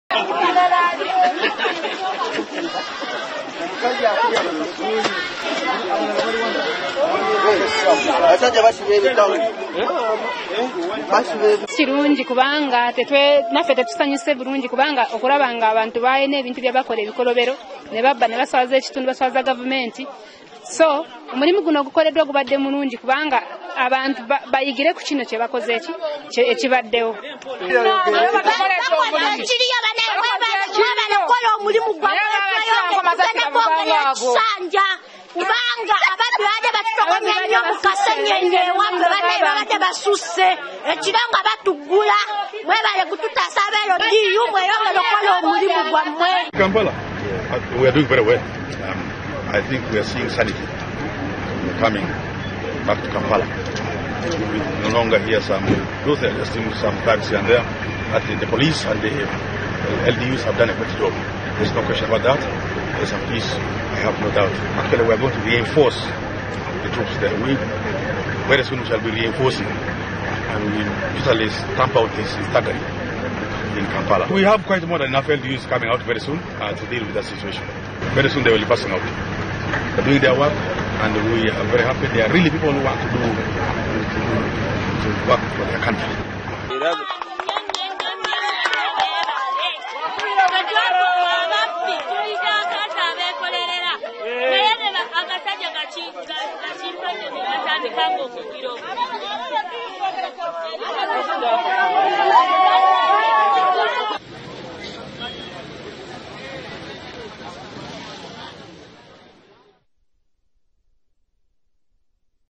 Gen Elwelu asiimye aba LDU abasse ababbi e Mutundwe
Aduumira amaye g’eggwanga ag’okuttaka Lt Gen. Peter Elwelu yebazizza nnyo aba LDU nabamaggye abakoze omulimu olunaku lweggulo nebatta abasajja bana abagambibwa okubeera ababbi b’emmundu.